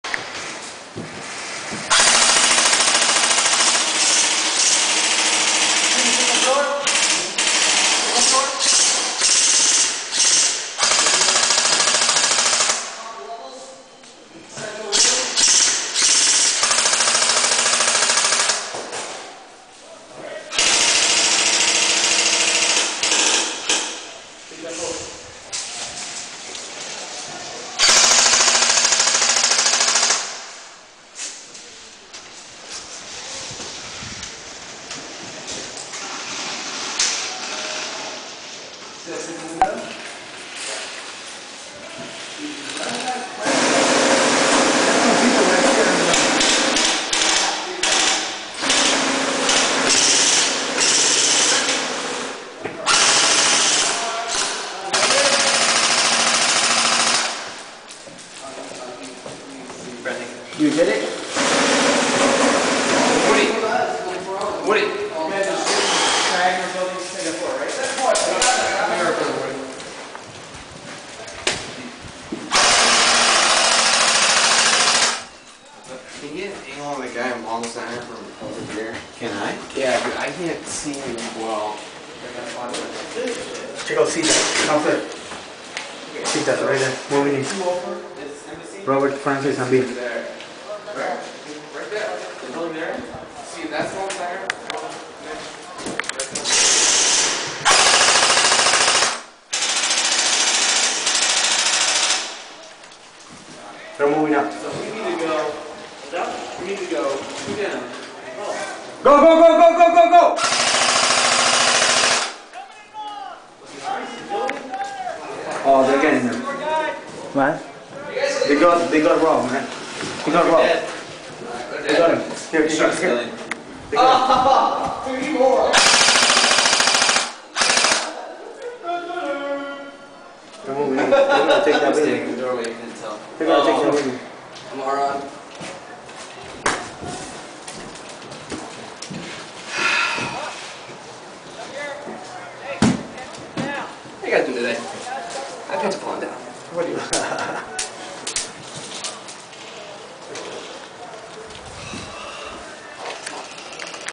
C.T.A.S. AIRSOFT # 1
Location: Fort Ord-California
C.T.A.S. Team members Richard(CA M15A4)and Diego(CA M249 PARA)providing suppressive fire.